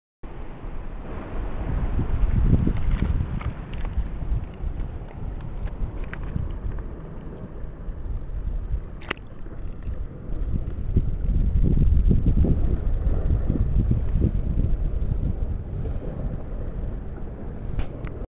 Feeding a mussel to a sea anemone
Feeding a mussel to a sea anemone at a tide pool in Santa Cruz